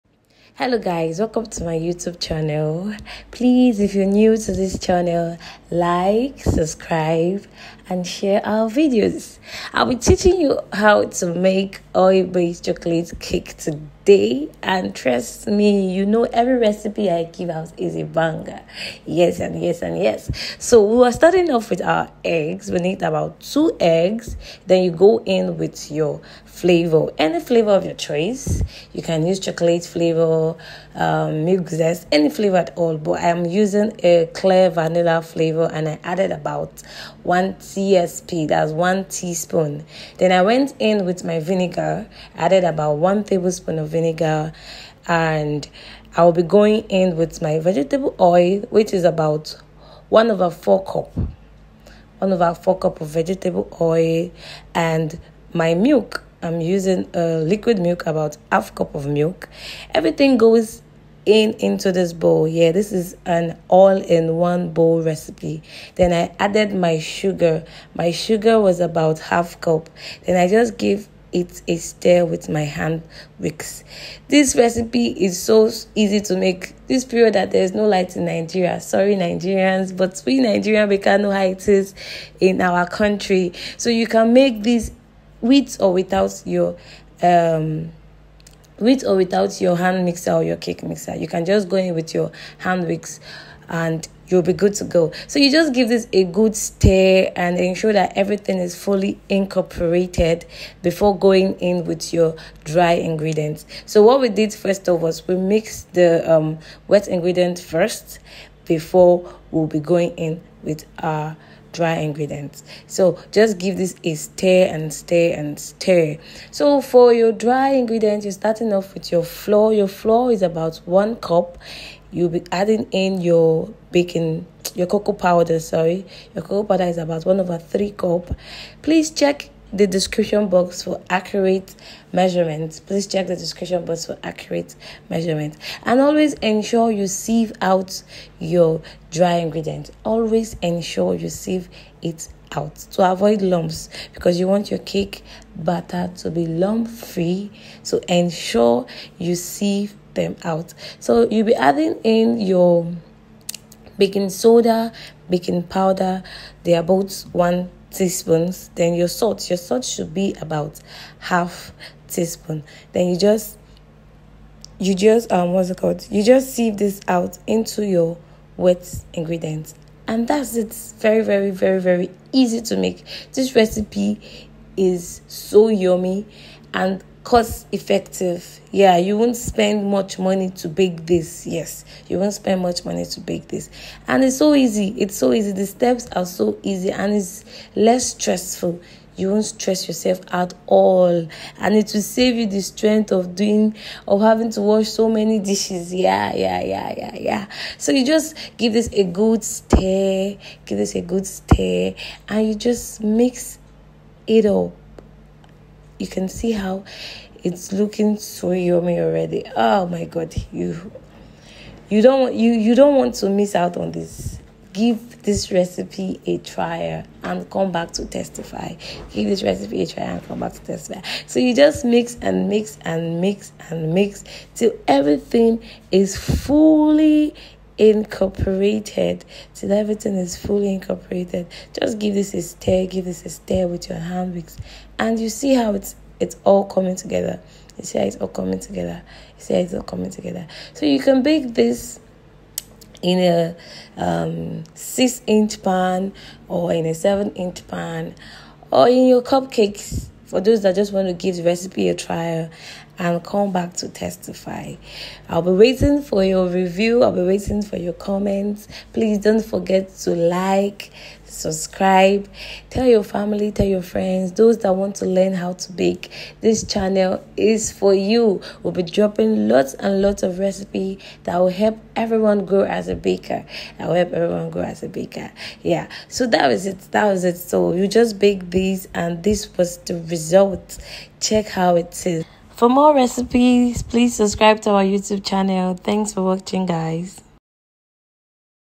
Mouthwatering Chocolate Cake Recipe | Quick & Easy Treat
In this video, we'll guide you step-by-step on how to create this delicious dessert from scratch. You'll learn the simple techniques to achieve that perfect lava flow and tips for getting the best texture. Whether you're a novice baker or a seasoned pro, this chocolate cake recipe is sure to become a favorite!

Recipe
2 eggs
1 tsp vanilla flavour
1 tbsp vinegar
1/4 cup veg oil
1/2 cup milk
1 cup sugar
1 cup flour
1/3 cup cocoa powder
I tsp baking soda
1 baking powder
1/2 salt